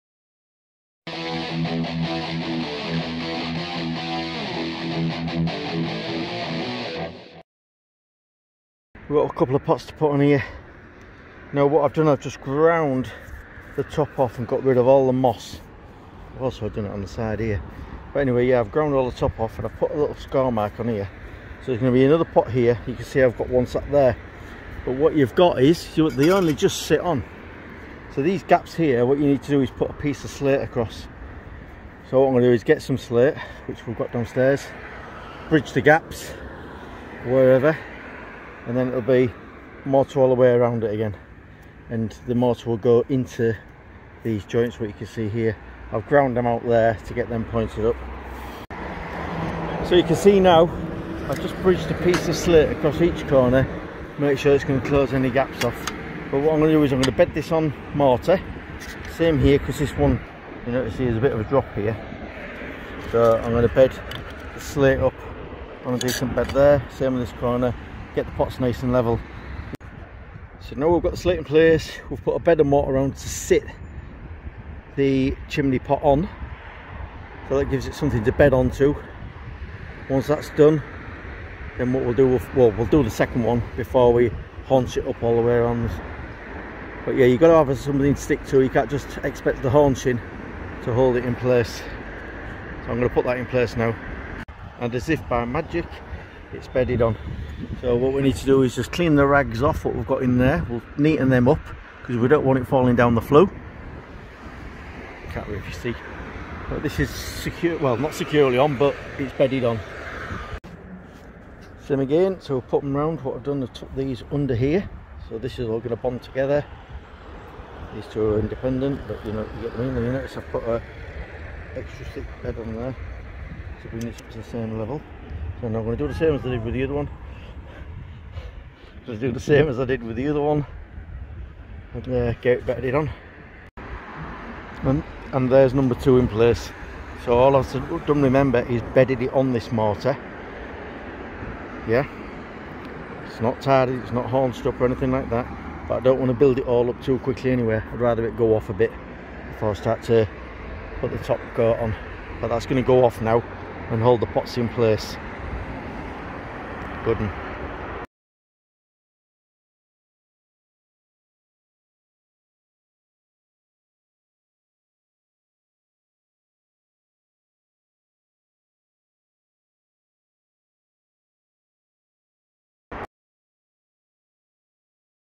INSTALL NEW CHIMNEY POTS LIKE A PRO!
In this video, we'll show you how to install new chimney pots like a pro!

Chimney pots are one of the most important parts of your fireplace system, and it's important to install them correctly to ensure optimal performance. In this video, we'll show you how to install new chimney pots quickly and easily, so you can get your fireplace up and running quickly and safely!
roof,chimney,pots,slate,Kingston Home Maintenance.